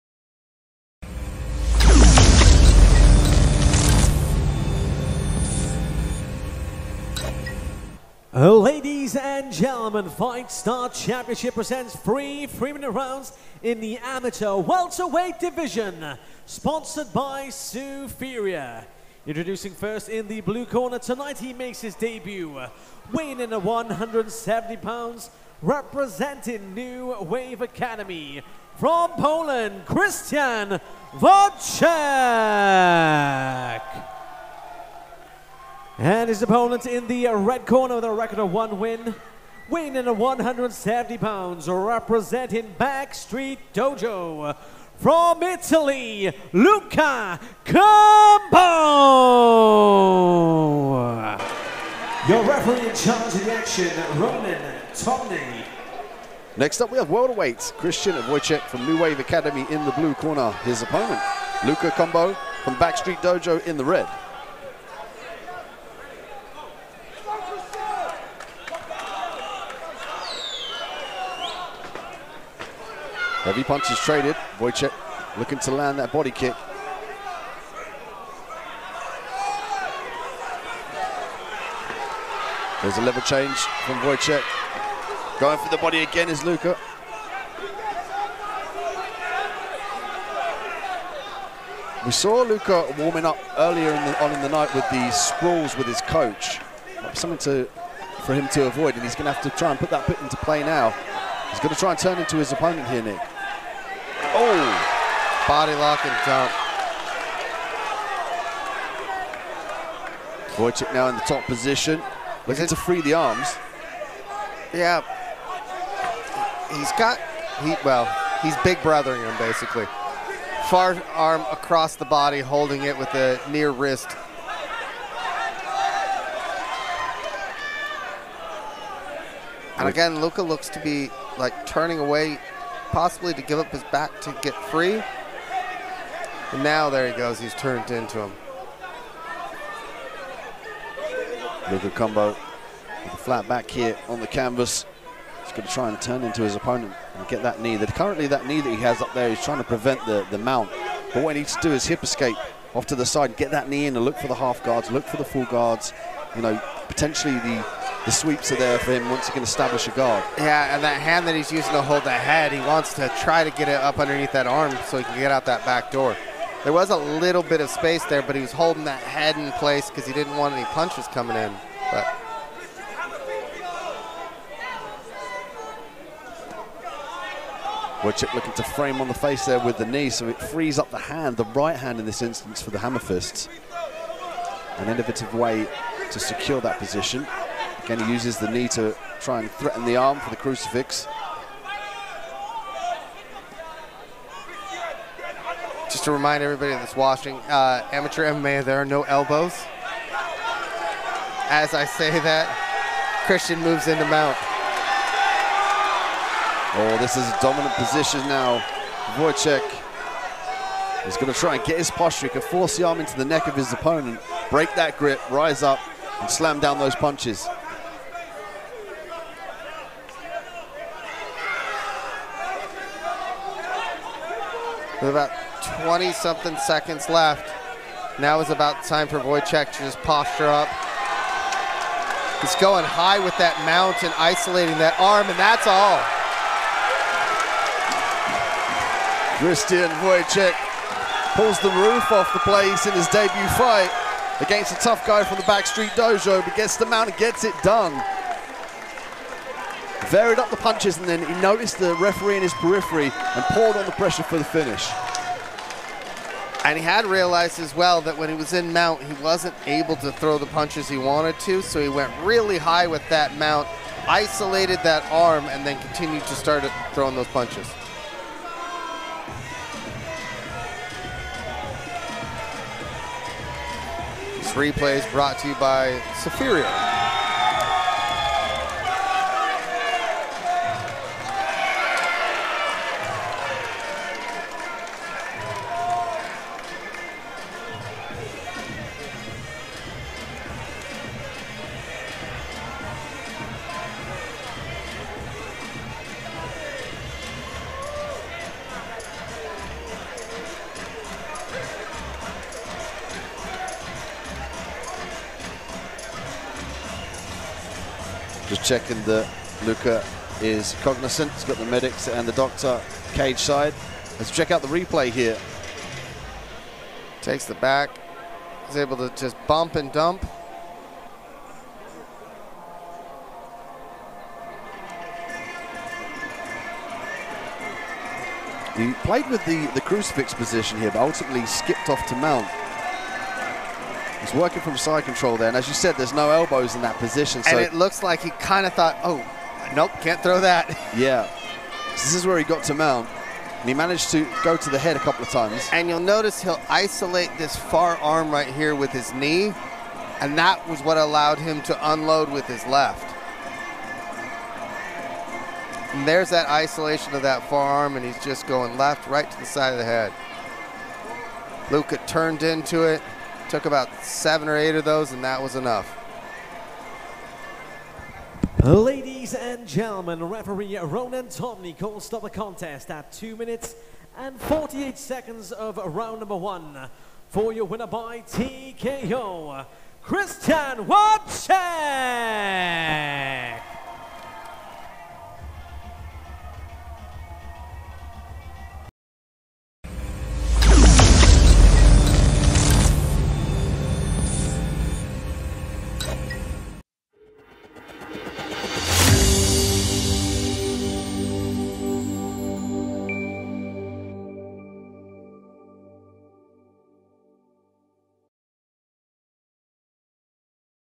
FSC22 | Krystian Wojcik vs. Luca Cumbo
𝐅𝐈𝐆𝐇𝐓𝐒𝐓𝐀𝐑 𝐂𝐇𝐀𝐌𝐏𝐈𝐎𝐍𝐒𝐇𝐈𝐏 𝟐𝟐: Fri. 15th July 2022, London.

9. Welterweight Bout: Krystian Wojcik 0-0-0 vs. Luca Cumbo 1-0-0